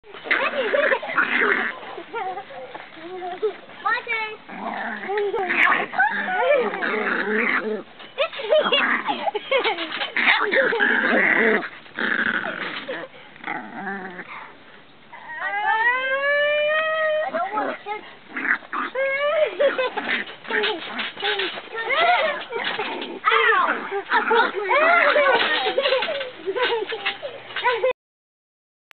Its Not The Size Of The Dog That Counts
Our Puppies liked to fight and the little one always got picked on. Well she learned real quick lol, thats its not the size of the dog that really matters because she took care of herself in this film an she hasnt grown much but she takes on dogs the size of wolves now. Haha.. She is a chih/doxen mix an her name is Ashley I will have to Upload recent videos because now she walkas around with this lil hop/skip thing and that is even more hilarious...